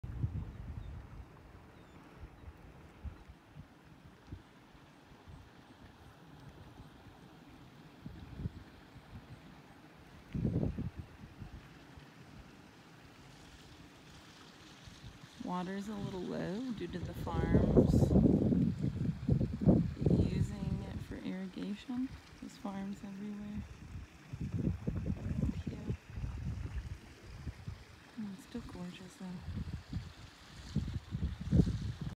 Video of Cauldron Linn BLM Dispersed, ID from Lady Wandlust L.
▶︎ Sweet spot
Ample amount of room, a handful of spots with various views,.. the river was really low due to farmers using it for irrigation,.. we chose to camp on the sandbar,.. no fire pit but the ground was tender enough to dig one out… no shade, lots of flys and hungry moths… people were picking stuff out of river but no one would give an actual answer to what it was they were picking up… cool rocks,..pretty tiny waterfalls.